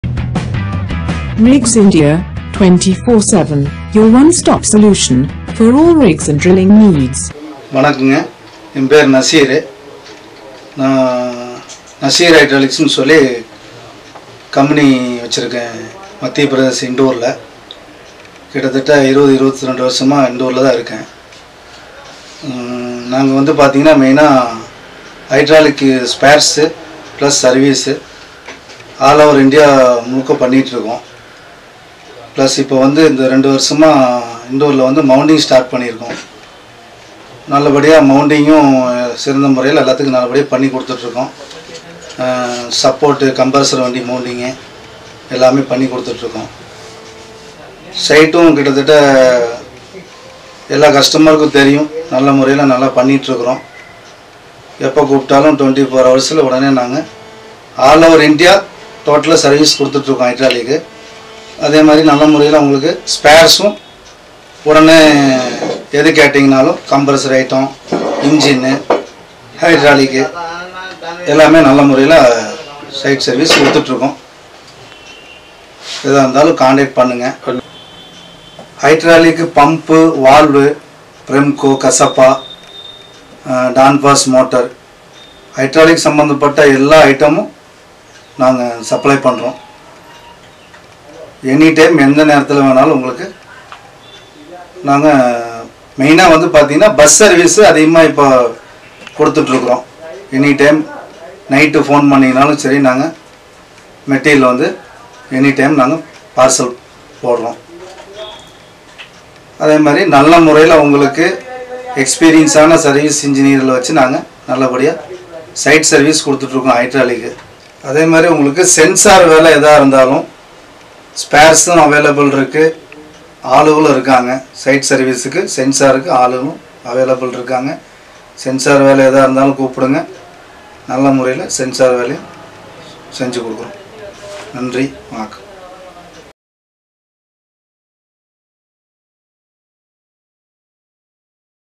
Hydraulic Spares + Service + Site Service அனைத்தும் ஒரே இடத்தில்
Mr. Nasir, Nasir Hydraulics ( Indore )
Running a Hydraulic Shop for the past 22 Years in Indore.

NATURE OF WORK
Hydraulic Spares
Site Service
Service
Rig Mounting
Support
Compressor
Bus Service
Sensor Work

We are doing "Site Services" and all other "Services" across all over India.
( Inc., SPARES / Compressor Items / Engine / Hydraulic )

We are also suppling all type of Hydraulic Items like Hydraulic Pump, Valve, Premco, Casappa, Donfoss Motor etc.,

In addition, We are doing BUS SERVICE ( Even in night time we'll parcel the material for our customer )

We are doing proper Hydraulic Site Service with experience Engineer.

SENSOR WORK : Available Workers for SITE SERVICE & Sensor Work.